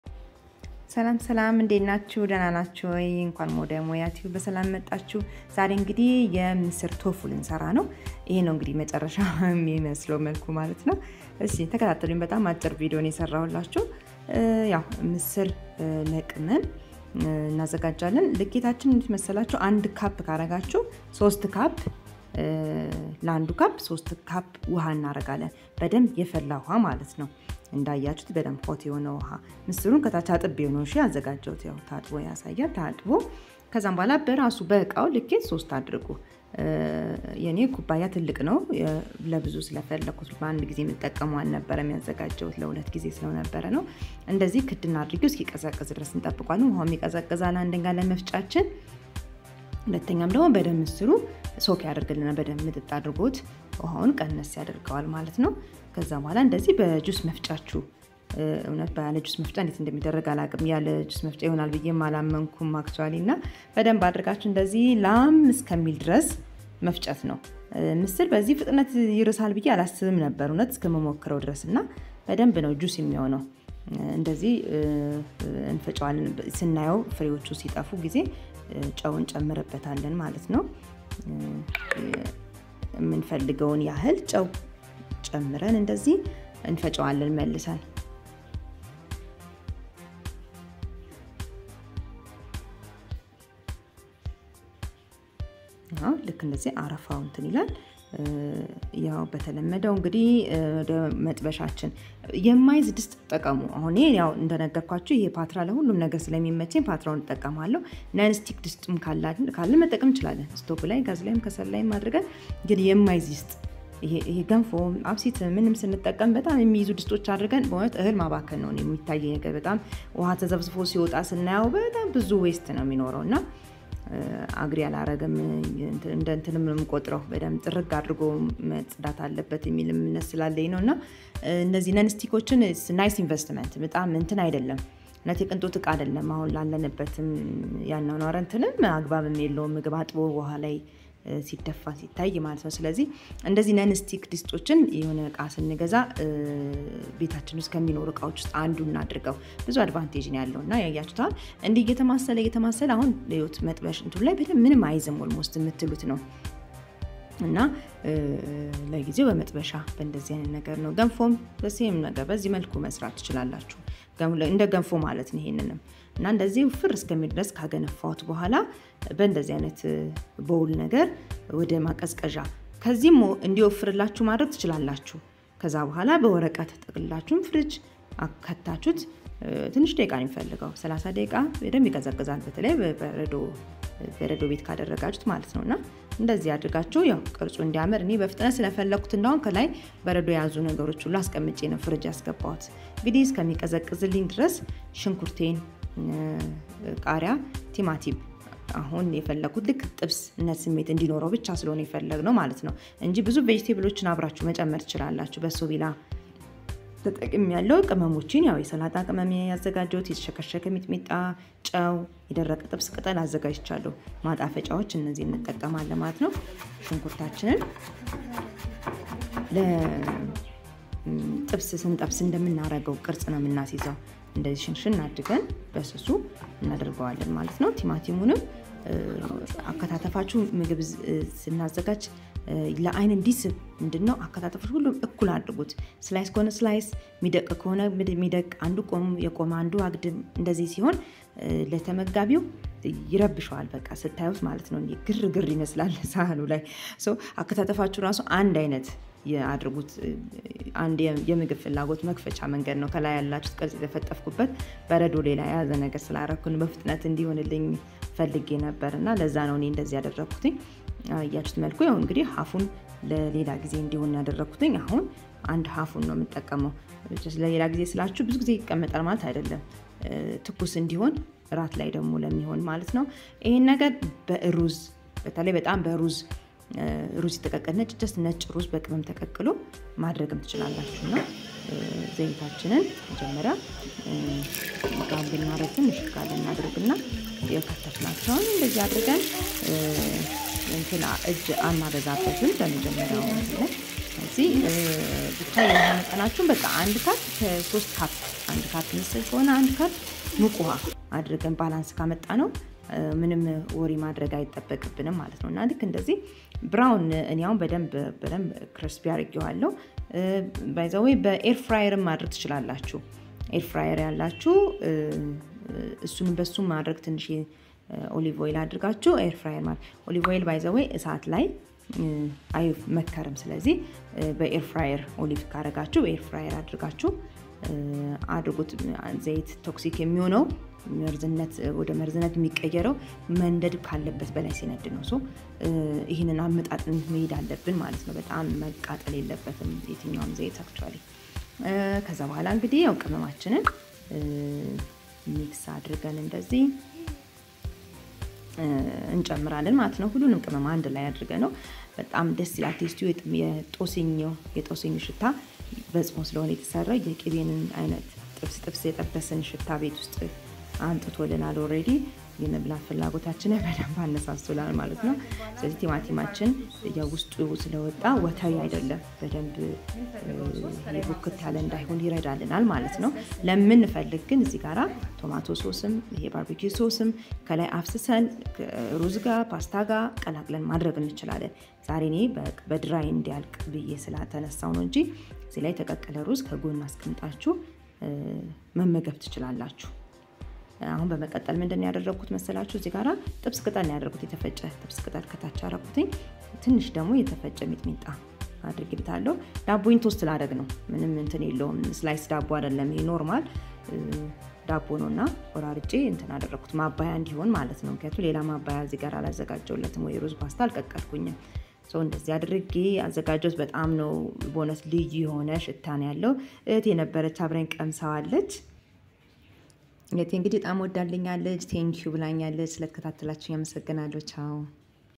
ምስር እንዲ ዘንጣ😀😀How to make tofu 100% Vegan!!!@moyatube755
how to make Tofu by lentil@moyatube755
ሞኩሩት ትወዱታላቹ።
what we can eat while fasting
all we need is
1. ምስር 1 ካፕ
2. 3 ካፕ የፈላ ውሀ
3. ቲማቲም
4 ሽንኩርት
5, ቃርያ
6 የጥብስ ቅመሞች
7. የተቀቀለ እሩዝ